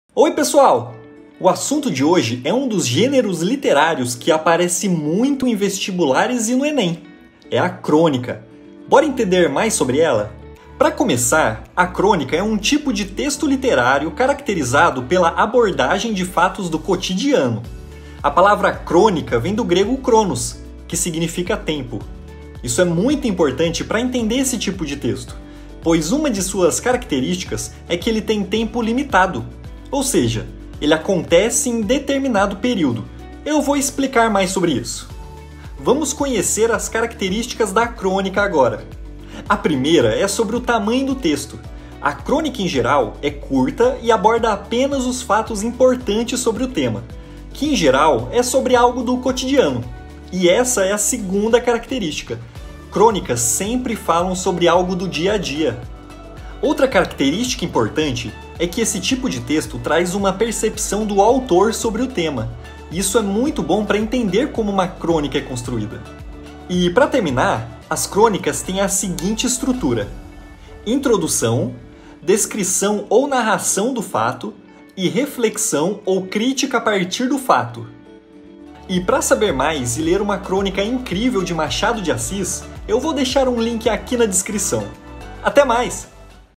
Tudo que você precisa saber sobre Crônicas em menos de um minuto | Cola Aqui ep. 21
O assunto de hoje é um dos gêneros literários que mais aparece em vestibulares e no Enem: a crônica. Bora entender mais sobre ela? Cola aqui!

Procurando por bolsa de estudos de até 80%?

Quer saber mais sobre esse tema? Vem cá